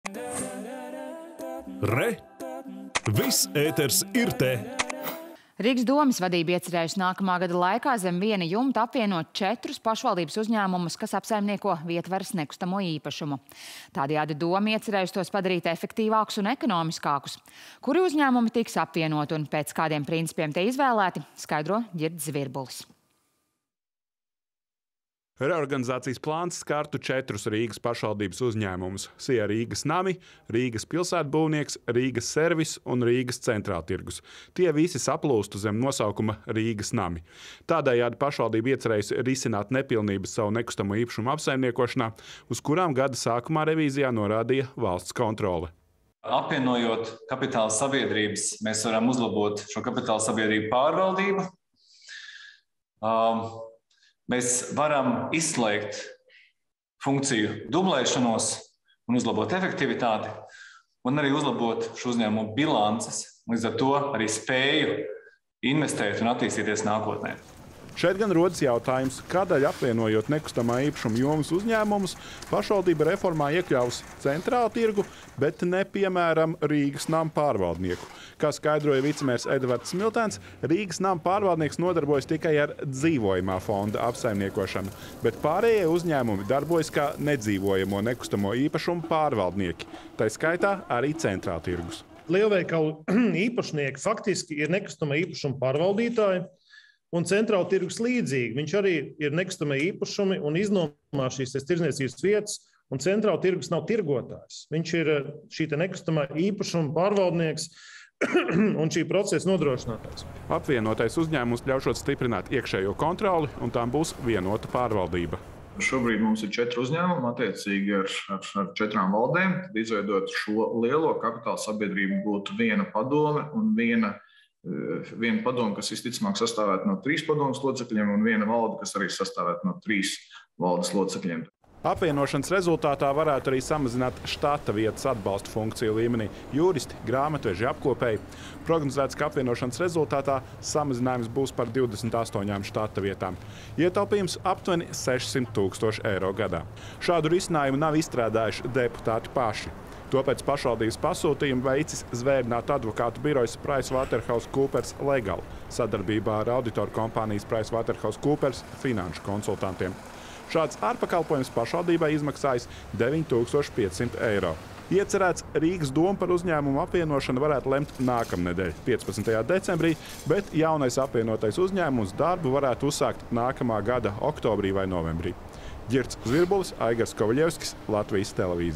Rīgas dome plāno apvienot uzņēmumus
Rīgas domes vadība iecerējusi nākamā gada laikā zem viena jumta apvienot četrus pašvaldības uzņēmumus, kas apsaimnieko pašvaldības nekustamo īpašumu. Tādējādi dome iecerējusi tos padarīt efektīvākus un ekonomiskākus. Plašāk stāsta Ģirts Zvirbulis.